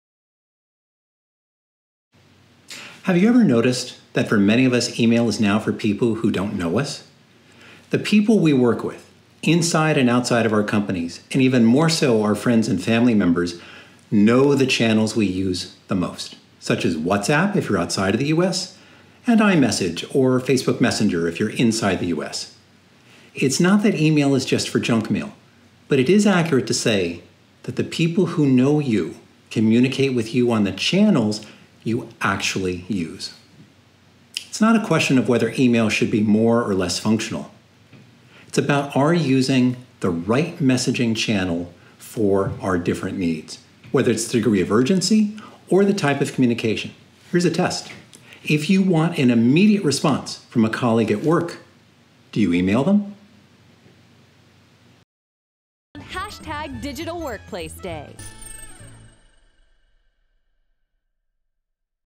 Do you want to see email be more functional or should it try to do less
Ken Herron, Chief Marketing Officer, UIB. Panelist for Session 4: The end of email – new collaboration methods for the digital workplace.

Question: Do you want to see email be more functional or should it try to do less

This session was a part of Digital Workplace Day, a 24-hour tweetathon on Oct 6th, presented by The Digital Workplace.